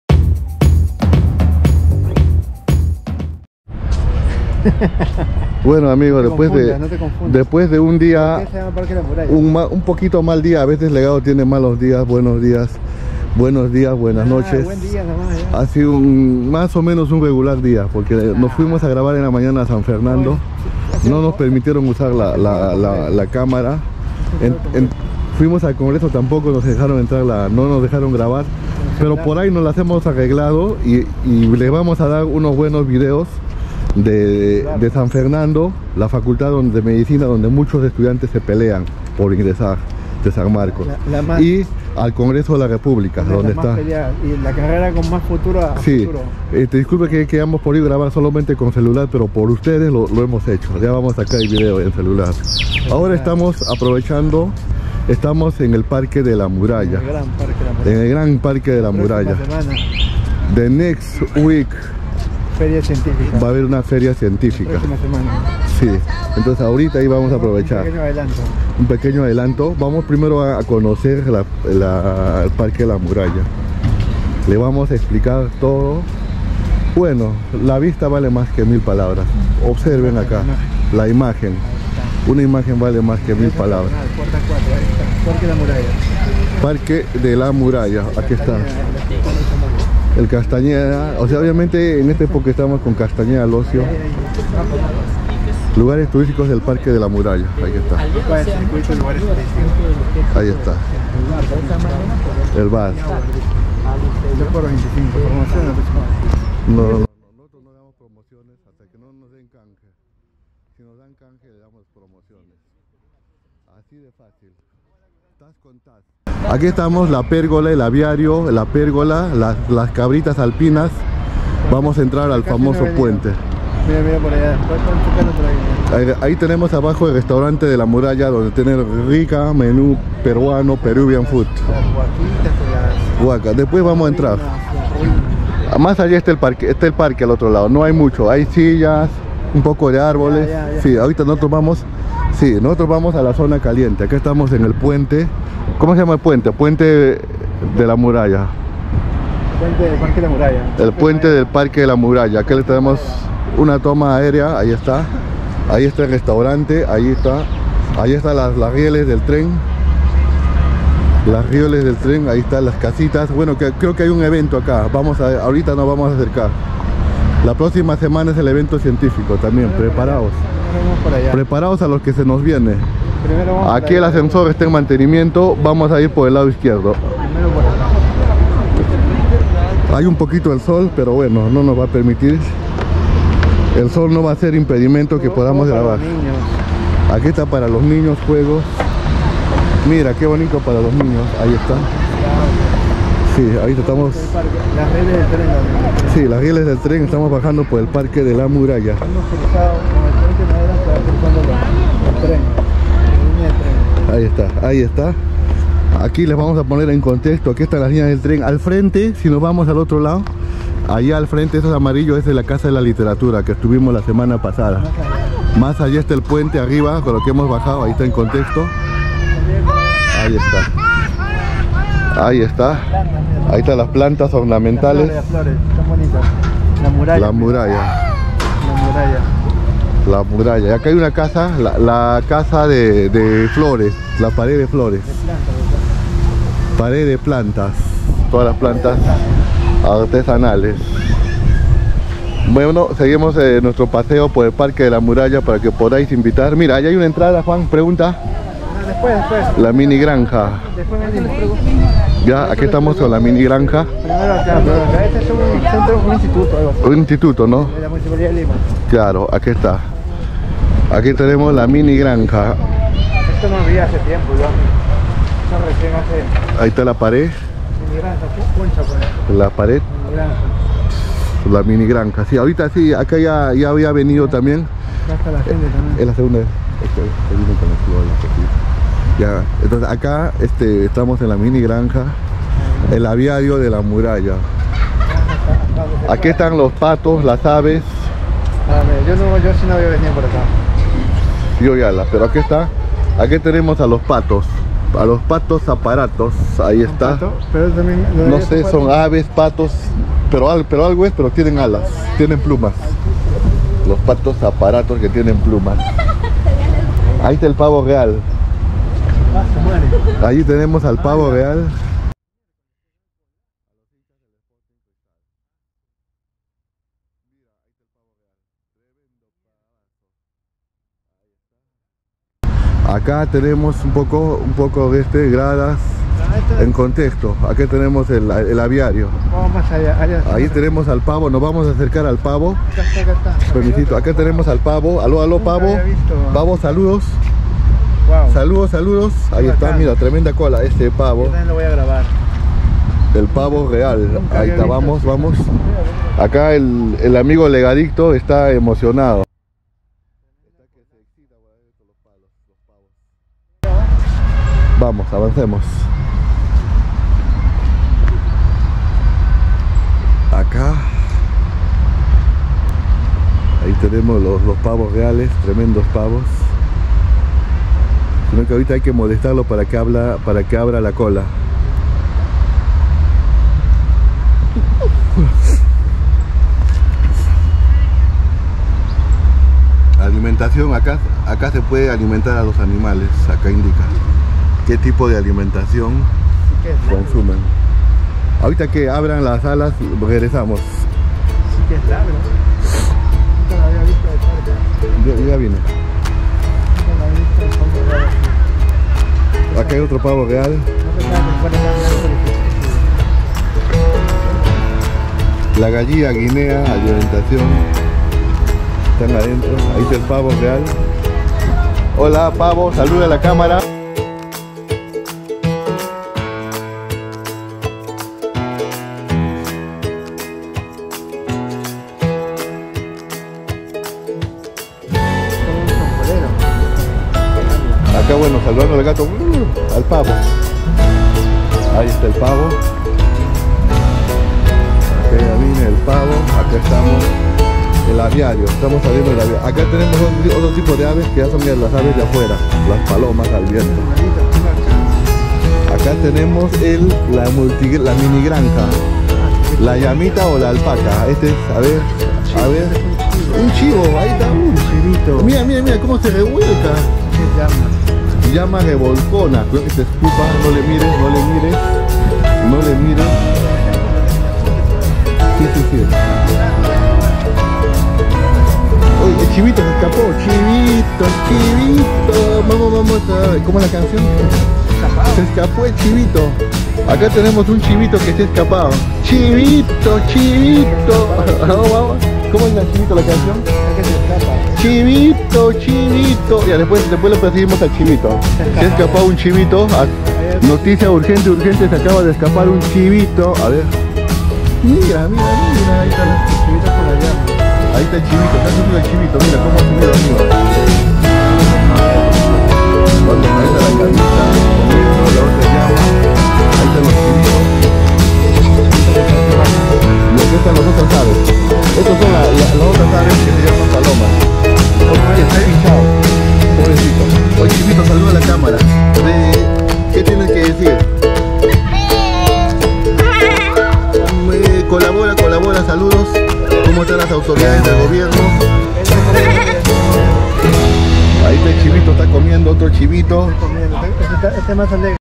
Visita al parque de la Muralla en el Centro de Lima - Peru
El parque de La Muralla es un parque público ubicado en el distrito de cercado en la ciudad de Lima capital de Perú. Dentro de su extensión se encuentra parte de la muralla de Lima. El parque está ubicado entre el río Rímac y el centro histórico de Lima  -  MURALLA PARK

También encontramos al Pavo Real.

🎈 La Muralla de Lima fue construida para protegerse de los piratas 🎋